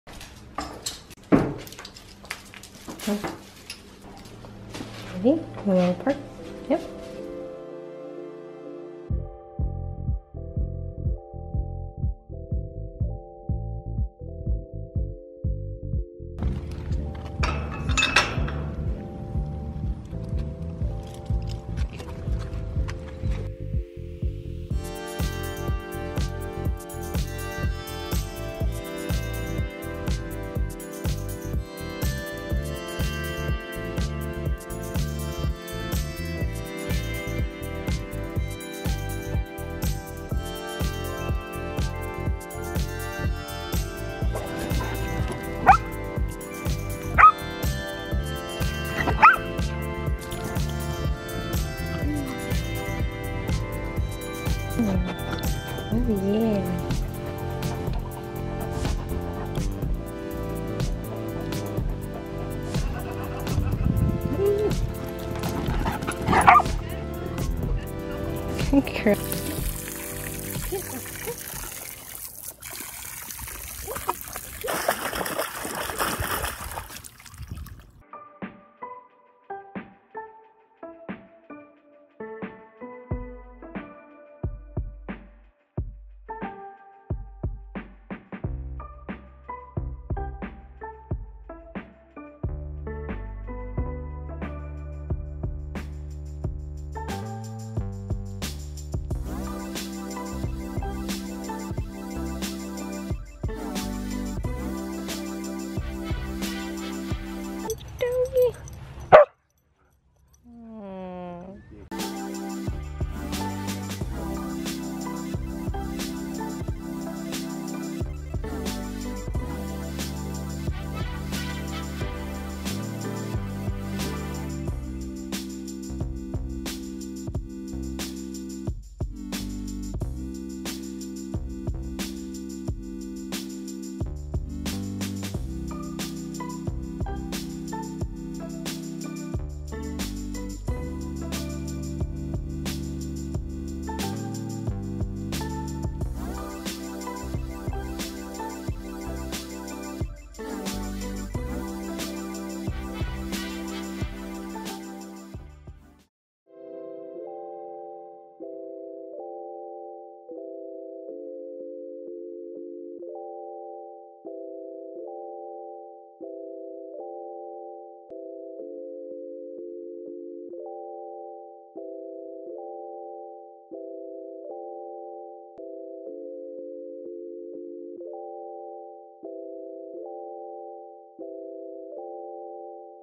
Let's go to the dog park 🐾
Here's a little dog park vlog to brighten your day :) We went to the Desert Vista dog park in Fountain Hills, Arizona. It's a very well-kept park and so spacious for hyper dogs like mine to get a high speed run out. Also easy to social distance since it's so big! For those asking, Kona is a 6 year old rescued Australian Kelpie.

FAVORITE DOG SUPPLIES*

MAIL
Sea Lemon
USA